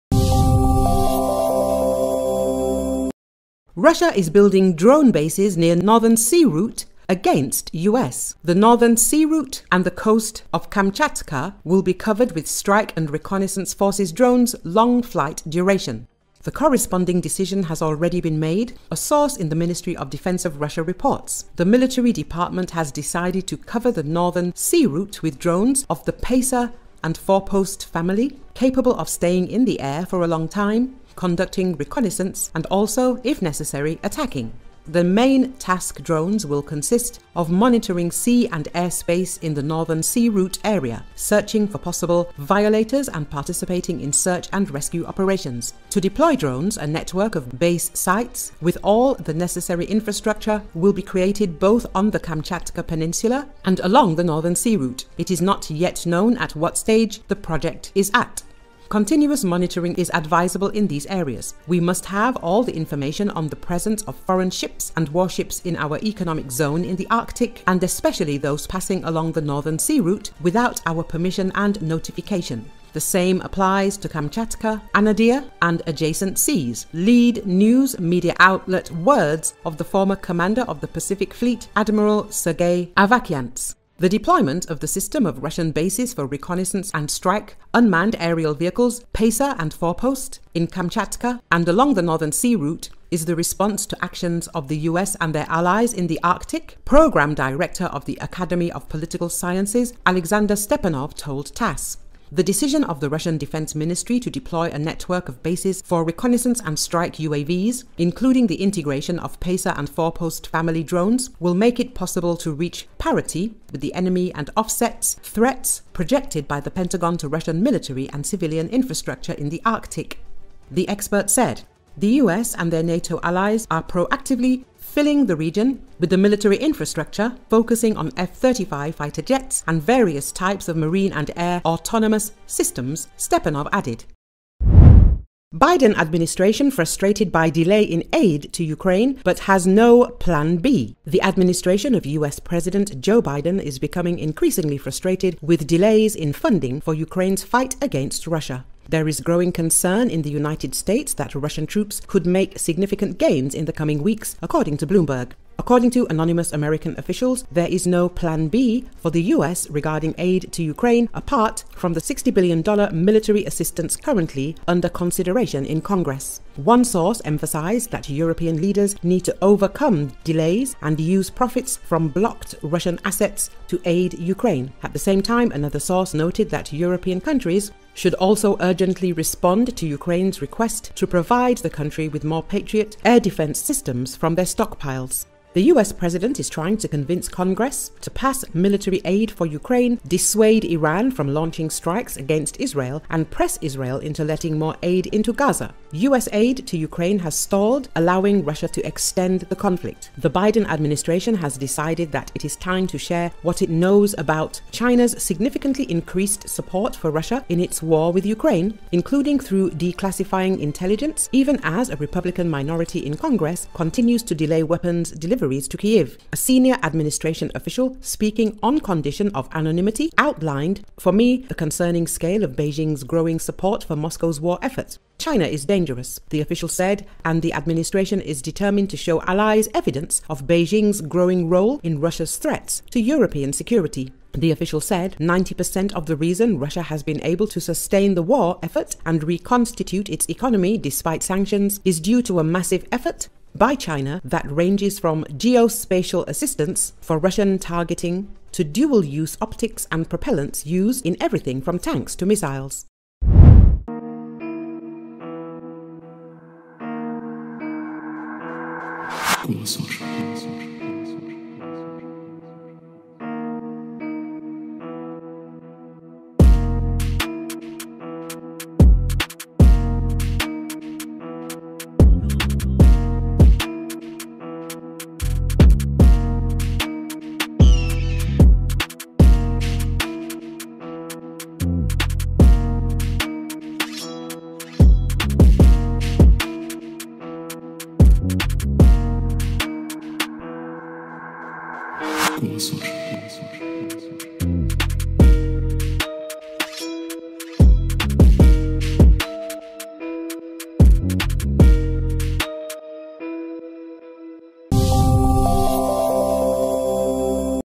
Russia is building a drone bases near Northern Sea Route against US
The Northern Sea Route and the coast of Kamchatka will be covered with strike and reconnaissance forces drones long flight duration. The corresponding decision has already been made, a source in the Ministry of Defense of Russia reports.
The military department has decided to cover the Northern Sea Route with drones of the “Pacer” and “Forpost” family, capable of staying in the air for a long time, conducting reconnaissance, and also, if necessary, attacking. The main task drones will consist of monitoring sea and air space in the Northern Sea Route area, searching for possible violators and participating in search and rescue operations.
To deploy drones, a network of base sites with all the necessary infrastructure will be created both on the Kamchatka Peninsula and along the Northern Sea Route. It is not yet known at what stage the project is at.
“Continuous monitoring is advisable in these areas. We must have all the information on the presence of foreign ships and warships in our economic zone in the Arctic, and especially those passing along the Northern Sea Route without our permission and notification. The same applies to Kamchatka, Anadyr and adjacent seas”, - lead "News" media outlet words of the former commander of the Pacific Fleet, Admiral Sergei Avakyants.
The deployment of the system of Russian bases for reconnaissance and strike unmanned aerial vehicles “Pacer” and “Forpost” in Kamchatka and along the Northern Sea Route is the response to actions of the US and their allies in the Arctic, program director of the Academy of Political Sciences Alexander Stepanov told TASS.
"The decision of the Russian Defense Ministry to deploy a network of bases for reconnaissance and strike UAVs, including the integration of Pacer and Forpost family drones, will make it possible to reach parity with the enemy and offsets threats projected by the Pentagon to Russian military and civilian infrastructure in the Arctic," the expert said.
The US and their NATO allies are proactively ‘"filling" the region with the military infrastructure, focusing on F-35 fighter jets and various types of marine and air autonomous systems, Stepanov added.